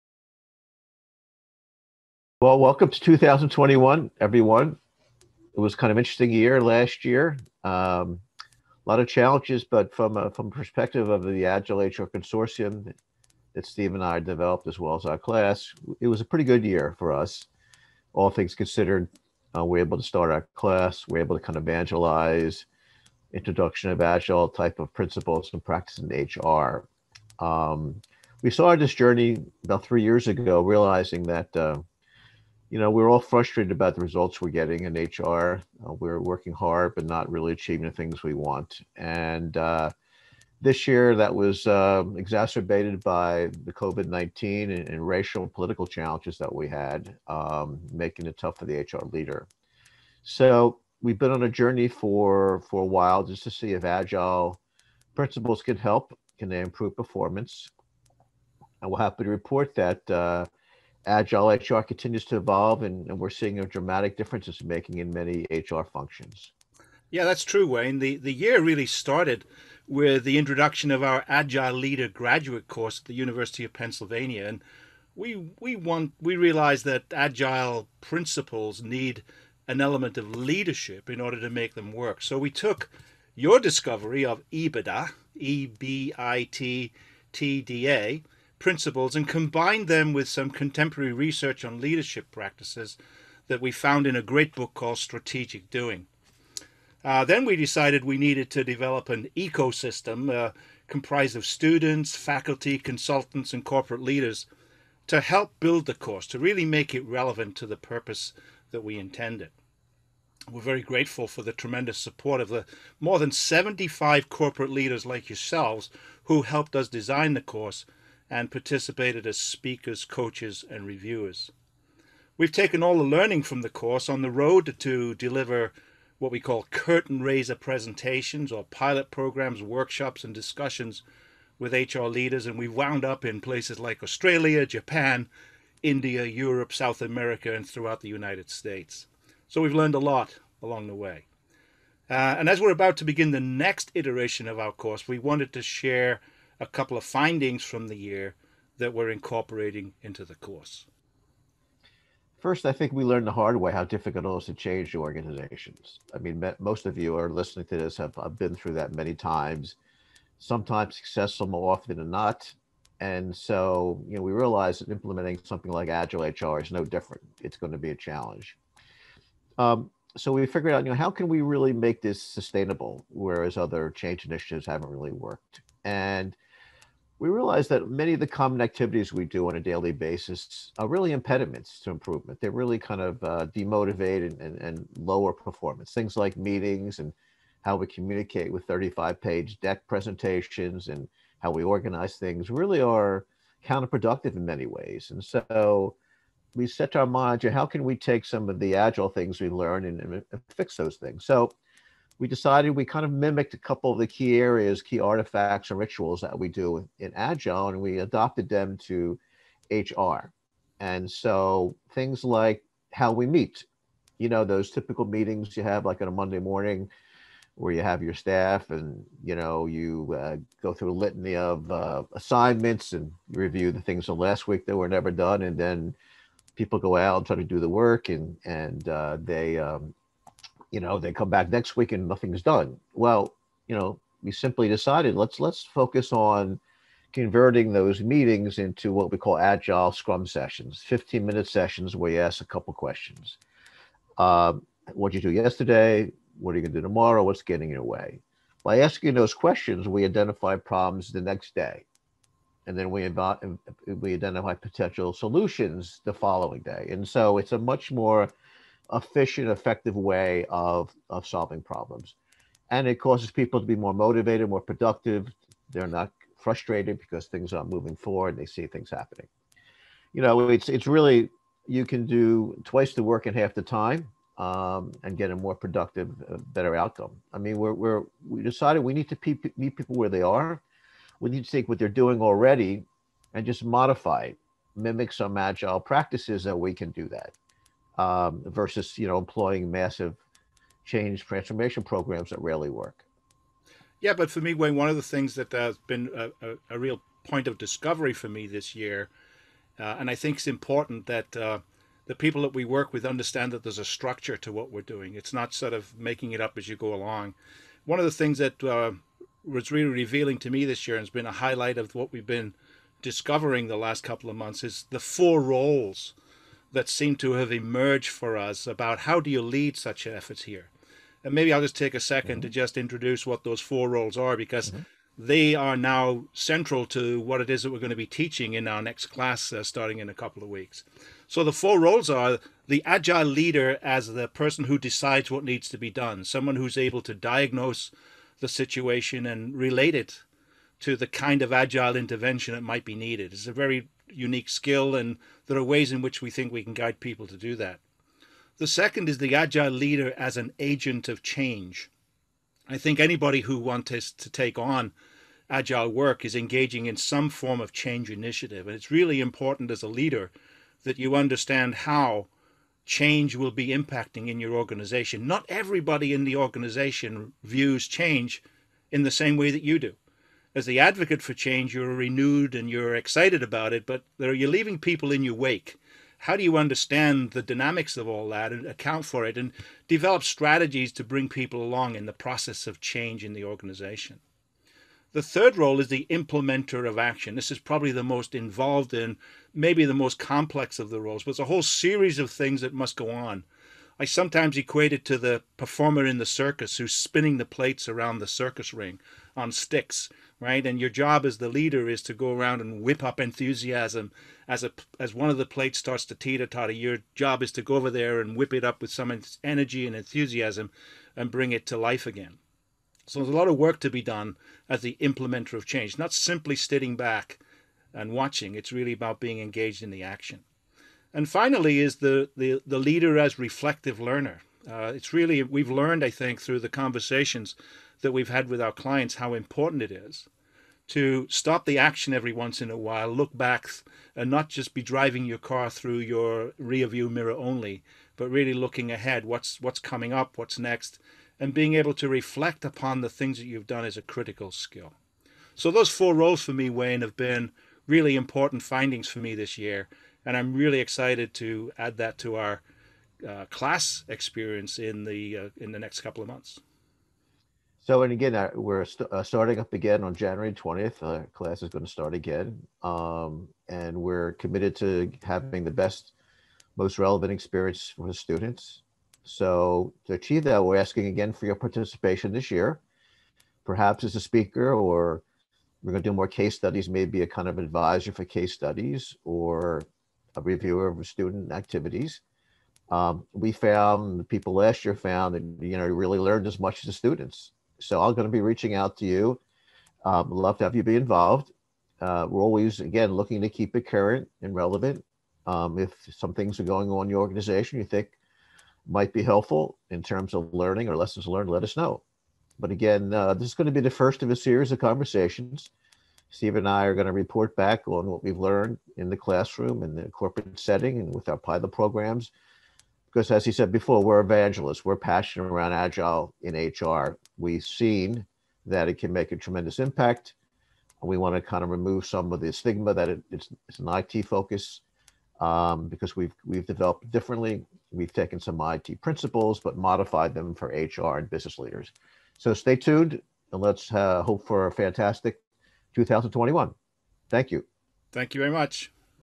Developing the Agile Leader and Their Organization Lesson Learned from the Classroom and Corporate
An exploration of the evolving role of agile leadership principle and practices in the transformation of HR while increasing the performance, agility, effectiveness and responsiveness of the function